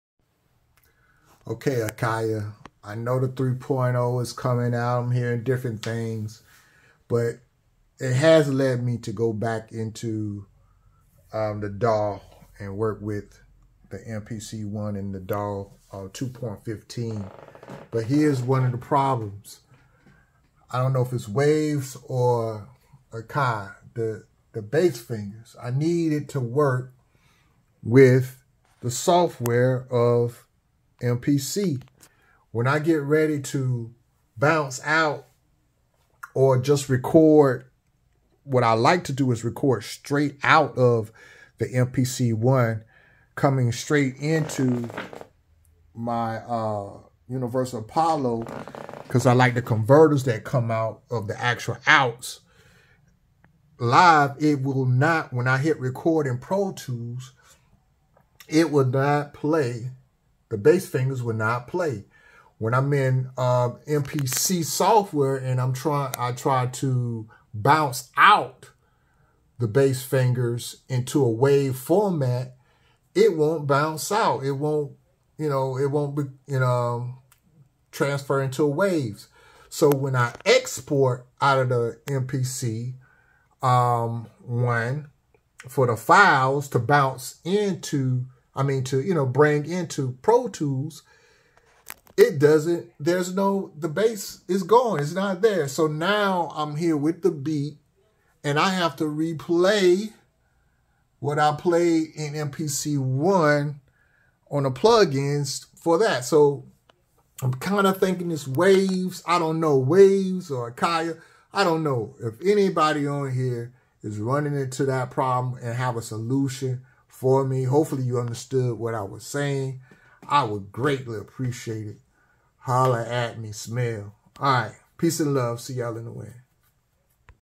Is it Akai or Waves? Bass Fingers plugin and MPC 2.15 not working properly.
Ok, I can't bounce it out, I can't record into protools coming from MPC software as my out into protools, I can't export it out. Now I have to replay the part in Pro tools. Help!!! mpcsoftware akai 3.0 mpc 2.15